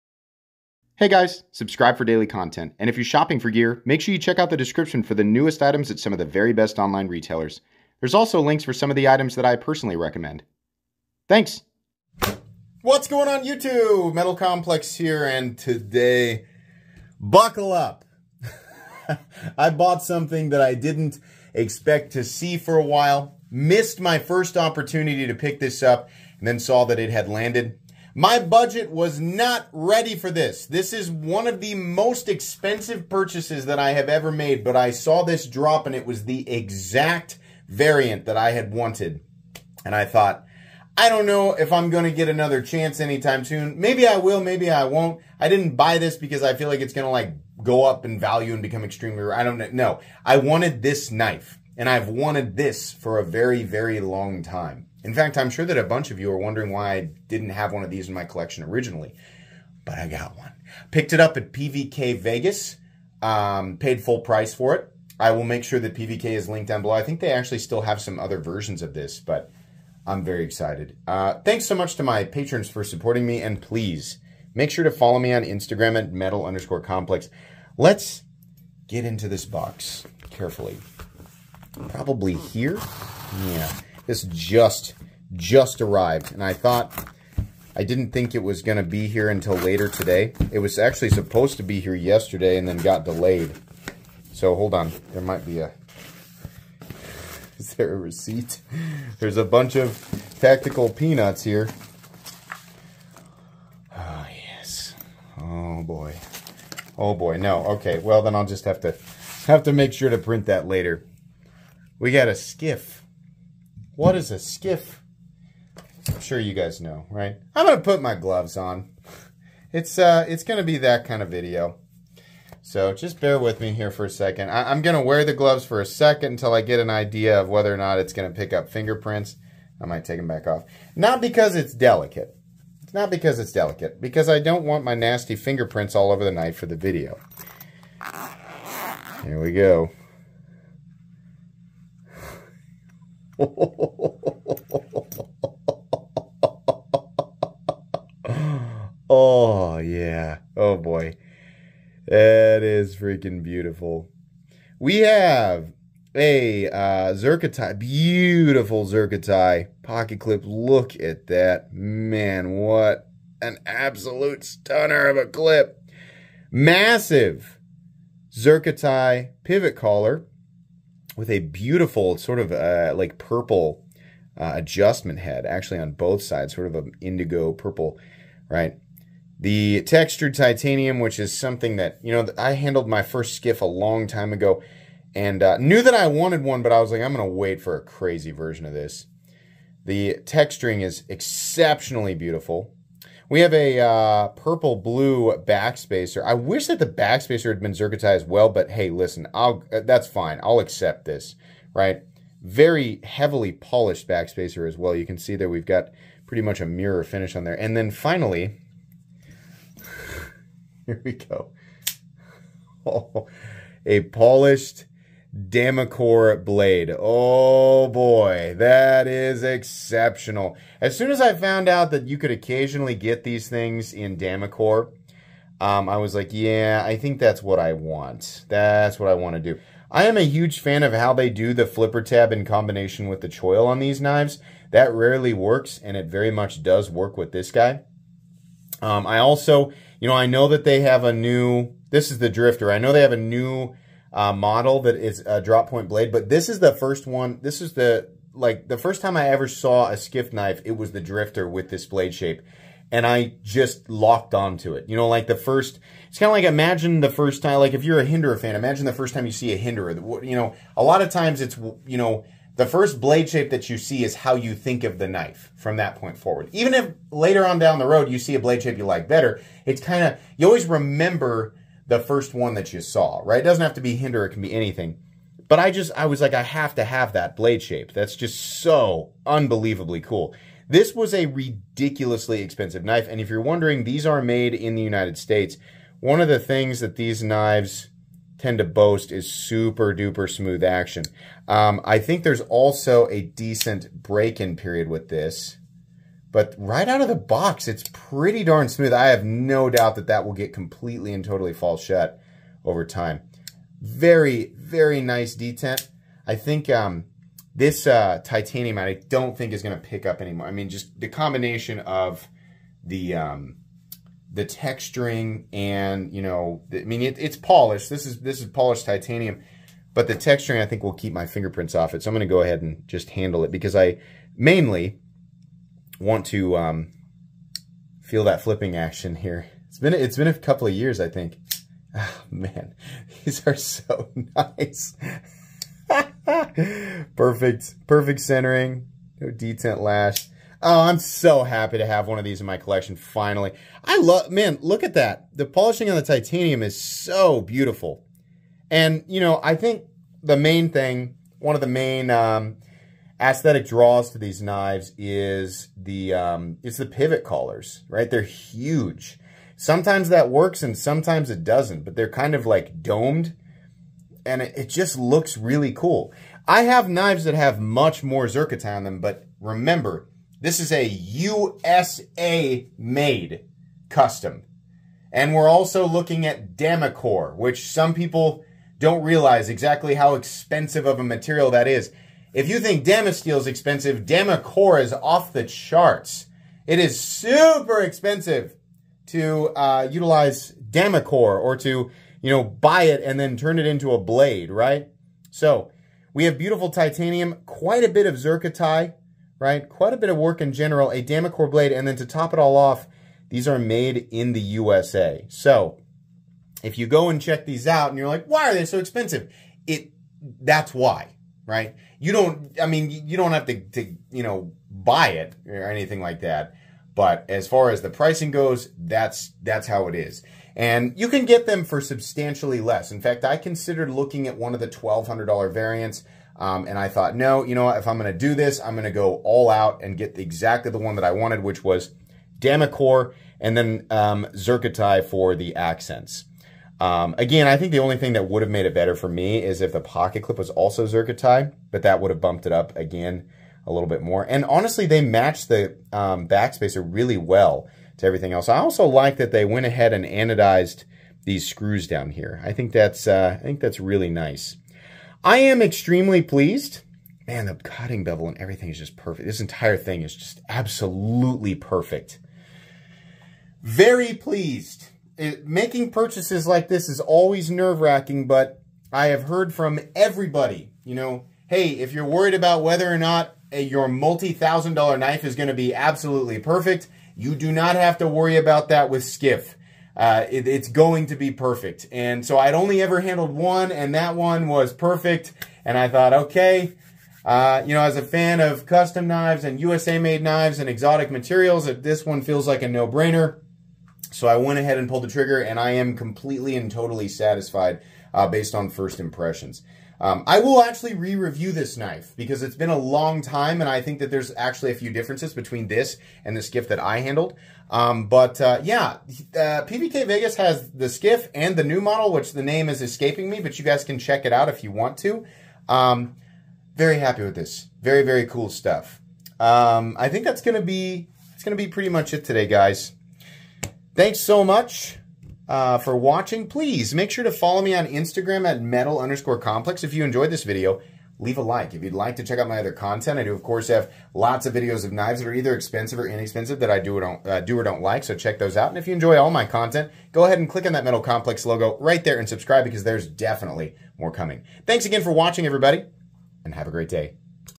😵 I CAN'T AFFORD THIS RIGHT NOW... But I Bought It Anyway 😵 - Knife Unboxing
Unboxing my new Skiff Drifter.

Check out Tools For Gents!